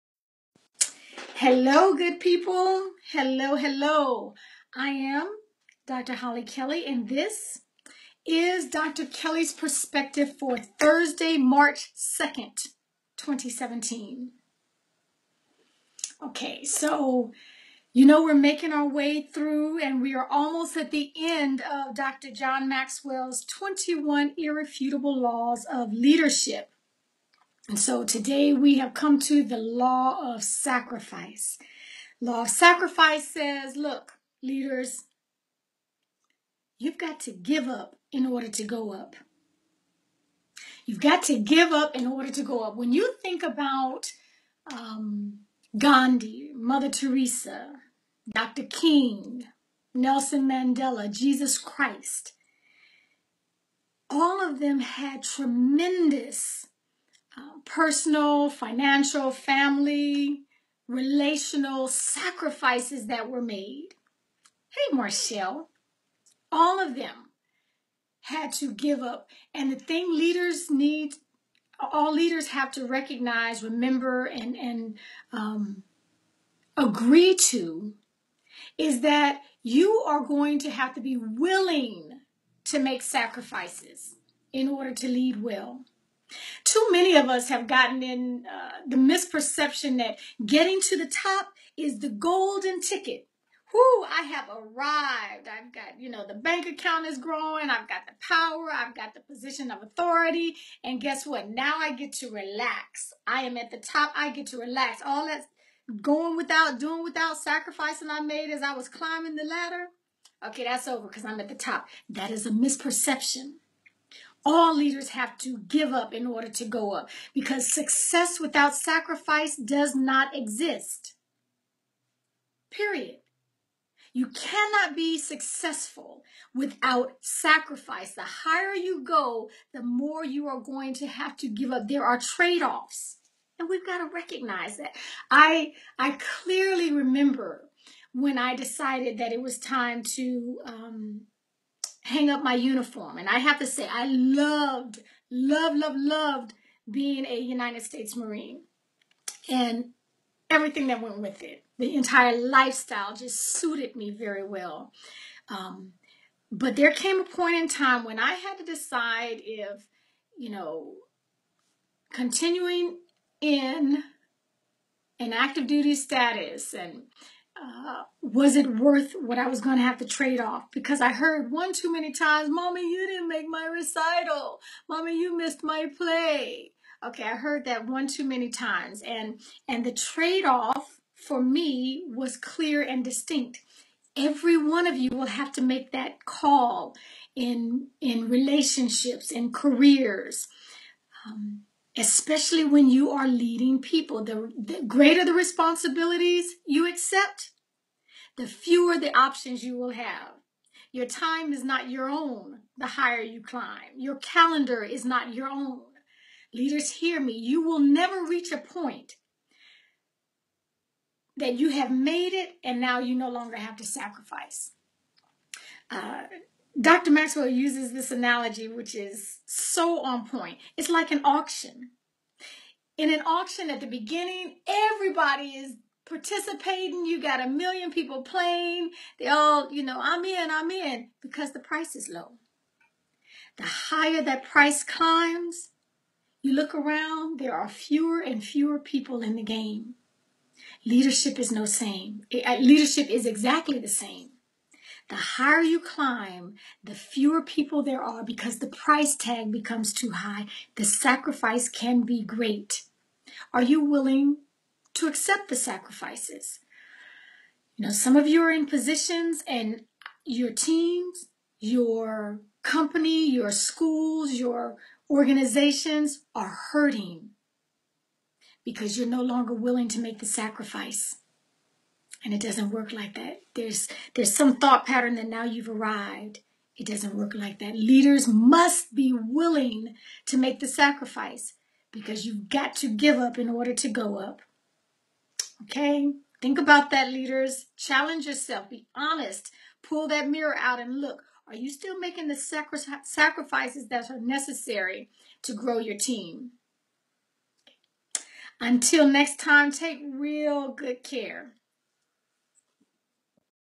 Dr. Hollie Kelly's Perspective: Law of Sacrifice 3.2.2017
Dr. Hollie Kelly shares her Perspective on Dr. John C. Maxwell’s The Law of Sacrifice from The 21 Irrefutable Laws of Leadership.  This law informs us that leaders recognize that to go up, they must go up because success without sacrifice does not exist.  There are trade-offs that must be made if an organization is to grow.  Dr. Kelly asks that you take a hard look to see if you have stopped making the sacrifices?  This video challenges you to continue paying the price in top leadership roles.   There is more information on how Dr. Kelly can help you do that

Schedule:
Tuesdays/Thursdays/Saturdays